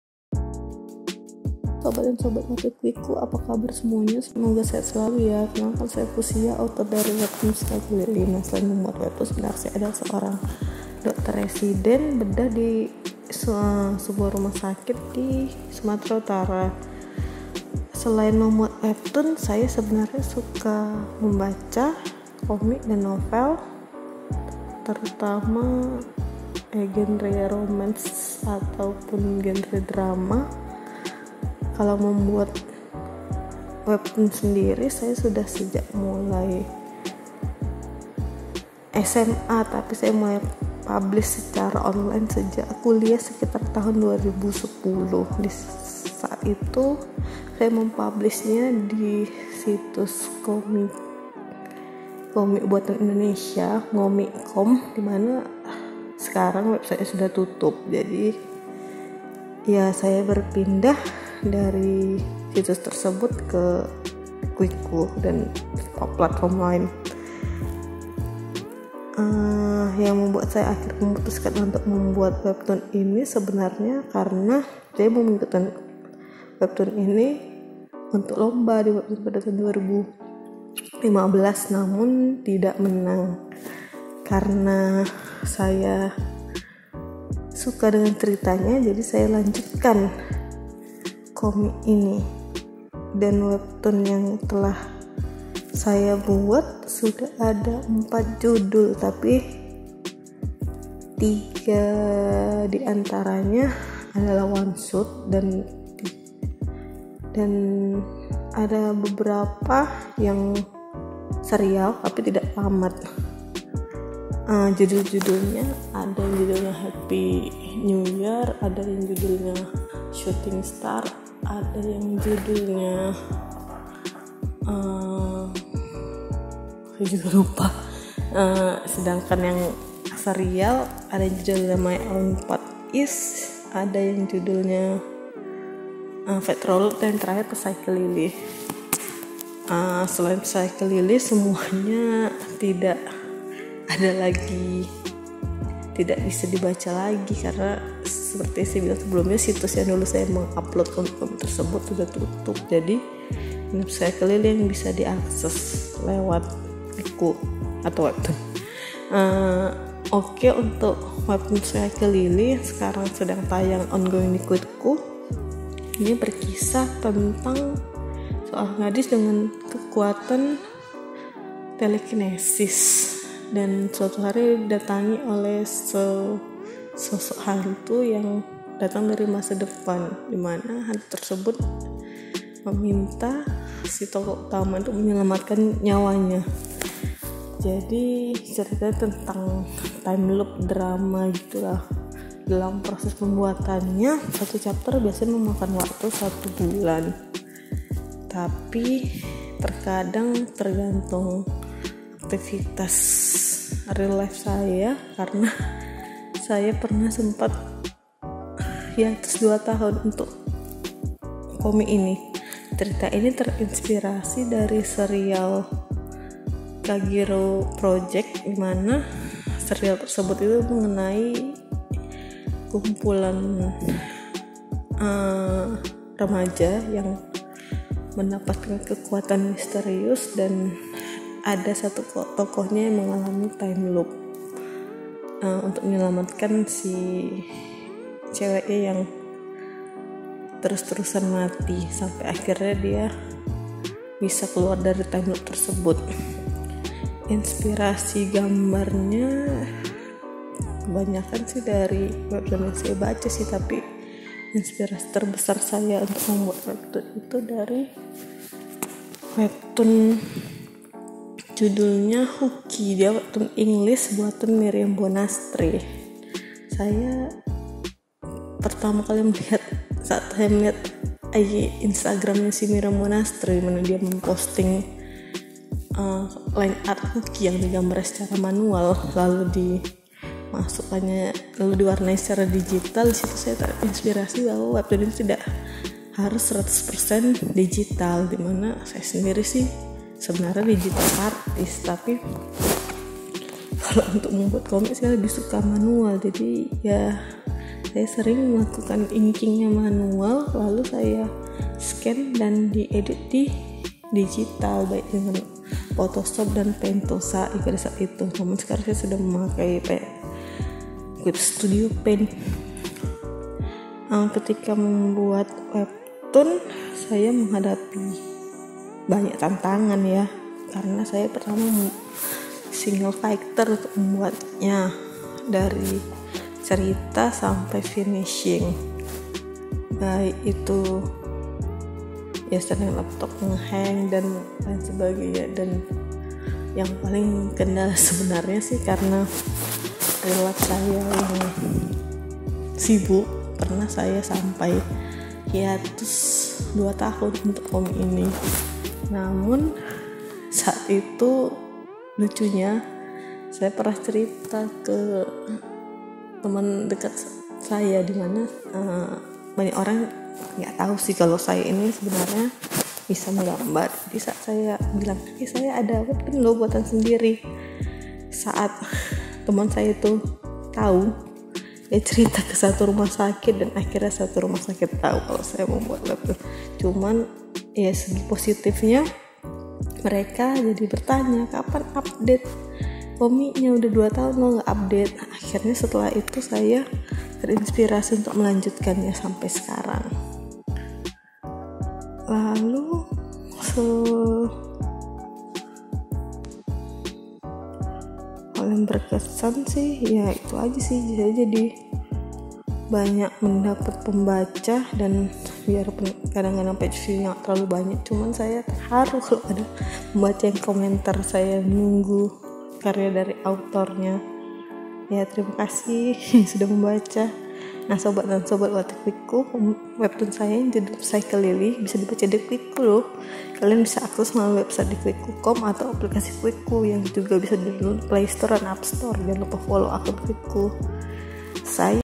Podcast Kwikku Bersama Fuchsia
Fushsia adalah author dari webtoon Psychelily

Tujuh tahun sudah sejak pertemuan pertamaku dengan 'setan kecil' yang membangkitkan kemampuanku terpendam yang sayangnya hanya melukai orang lain. Aku sudah hampir melupakannya hingga suatu hari datang seseorang yang juga melakukan kontak dengannya. Tapi seseorang itu, bukan manusia...